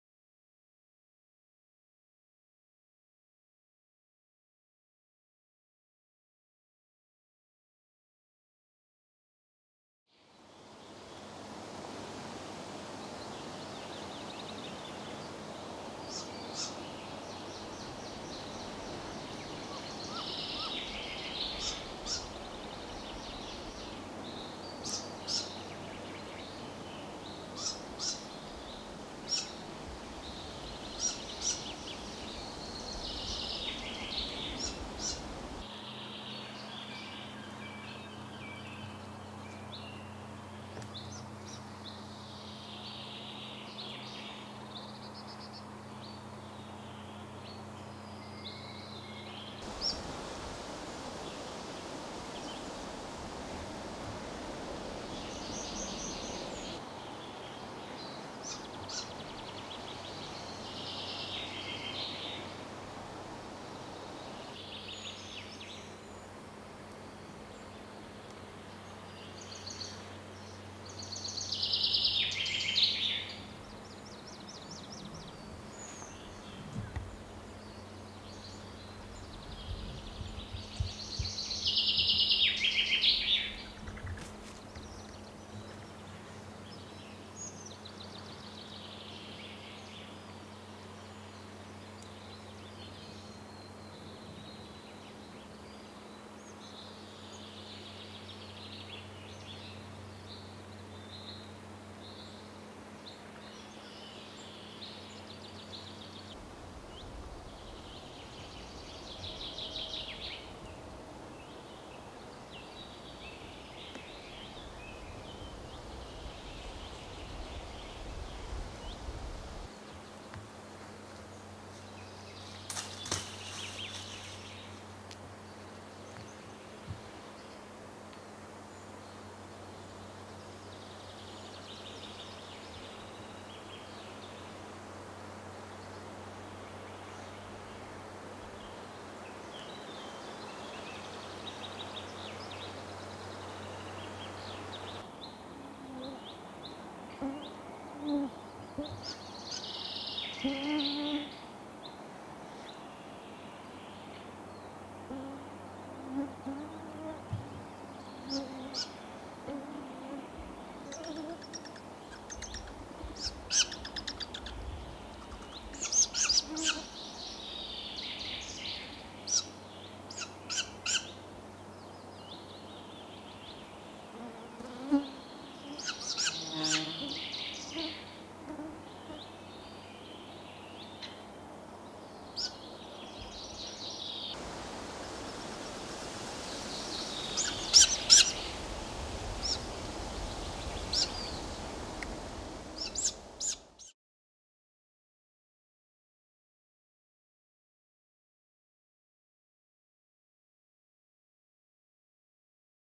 LAPPUGGLA Great Grey Owl (Strix nebulosa) Klipp - 2151
Lappuggla. Ungarna visar sig för första gången. 16-Maj 2010 i Västmanland. Great Grey Owl in Sweden,  AROS FILM Fotograf: Gunnar Fernqvist. Västerås. Sweden.  (Rf-187.)